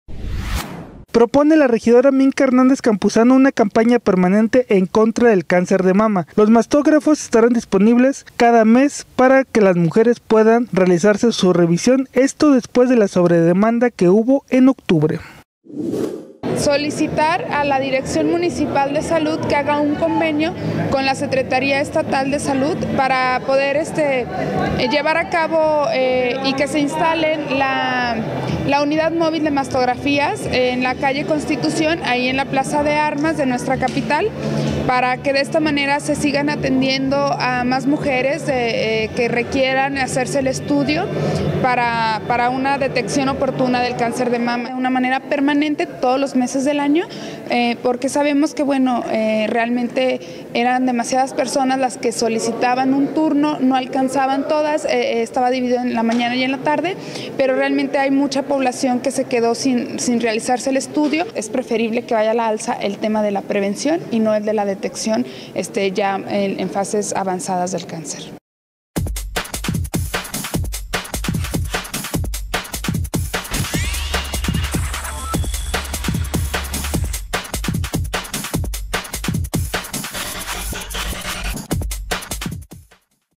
Cáncer de mama, segunda causa de muerte de mujeres
Pretenden que haya una campaña permanente de detección oportuna del cáncer de mama.

Se aprueba un punto de acuerdo por unanimidad para que haya una campaña permanente de detección oportuna del cáncer de mama, por lo que se pretende firmar un convenio con la Secretaría de Salud, para que los primeros diez días de cada mes se instale una unidad móvil de mastografía en la plaza de Armas de la ciudad capital, aseguró la regidora panista, Minka Hernández Campuzano, esto ante la sobredemanda que hubo en el mes de octubre y por ser la segunda causa de muerte de mujeres, entre 35 y 54 años de edad.